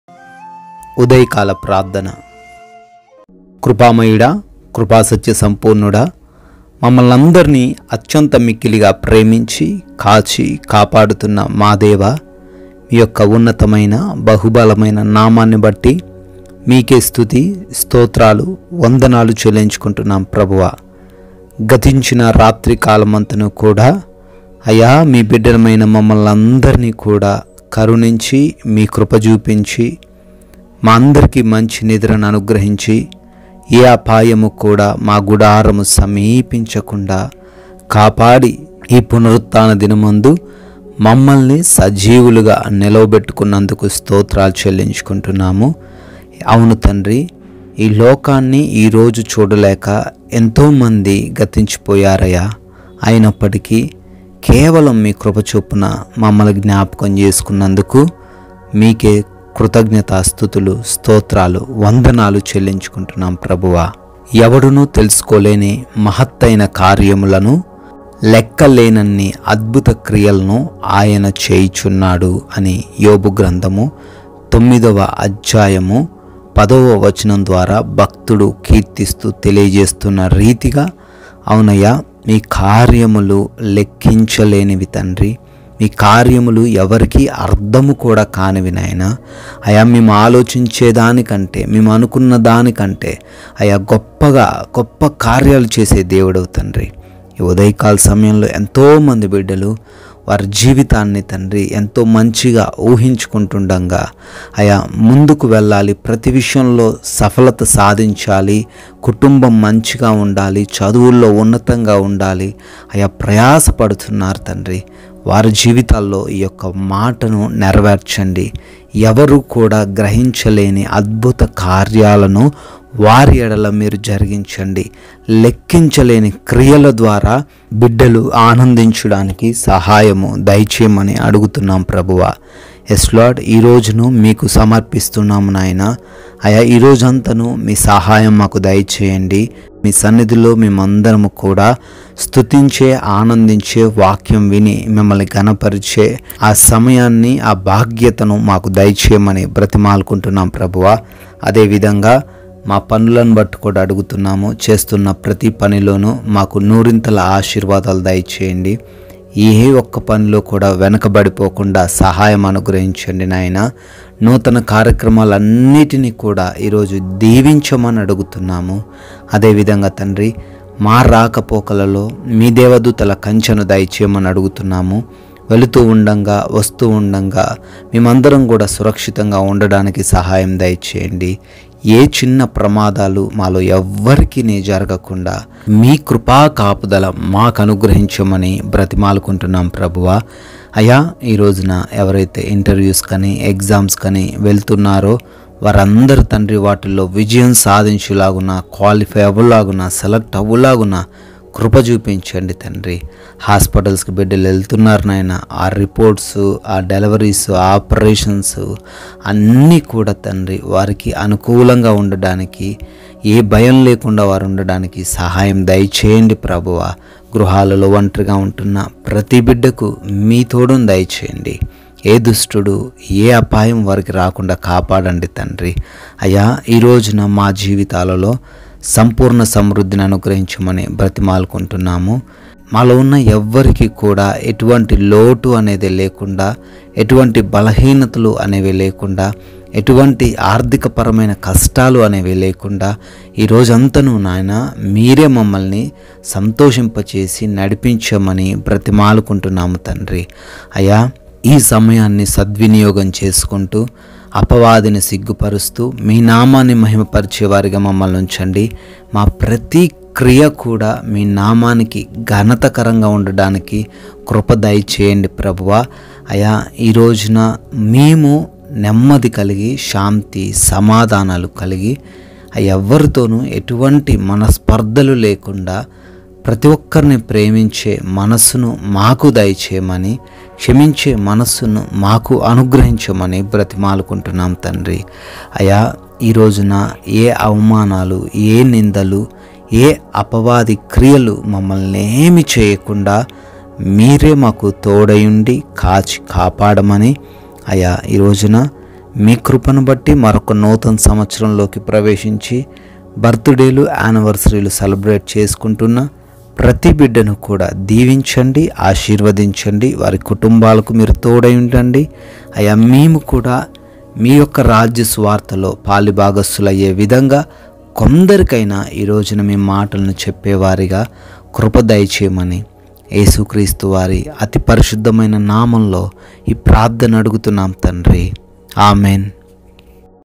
Morning Prayer 29/09/2024 /udaya kaala prardhana | Christ Miracle World
Morning Prayer 29/09/2024 |Prayer to start the day | udaya kaala prardna| Blessed day | Christ Miracle World

every one should do This morning prayer Morning Prayer

*Today's promise
*Praise and worship to God
* Morning prayer
* Night prayer